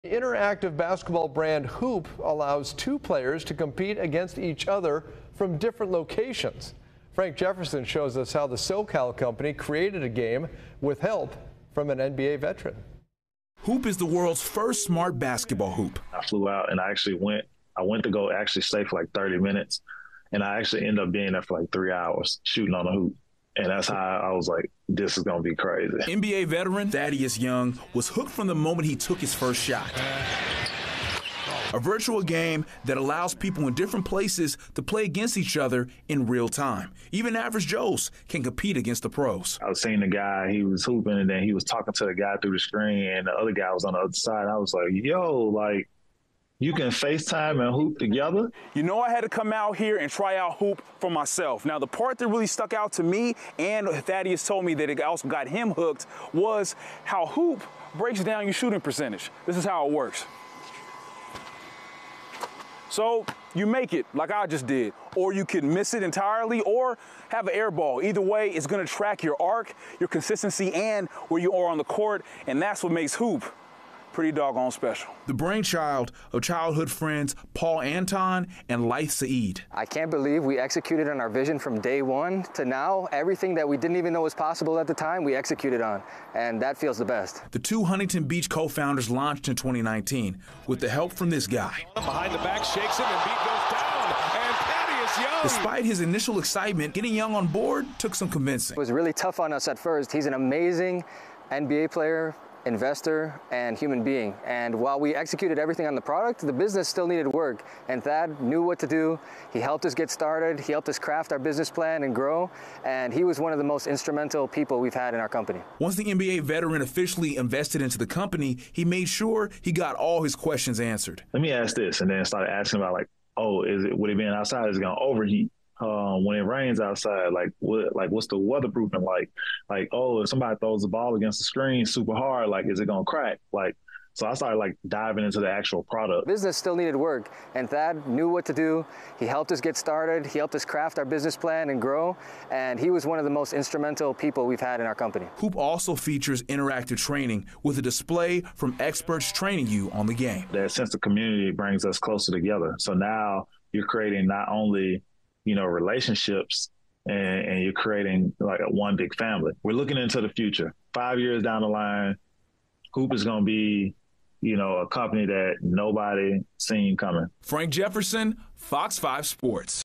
Childhood Friends Create High-Tech Hoop With NBA Vet
Huupe is the world’s first smart basketball hoop.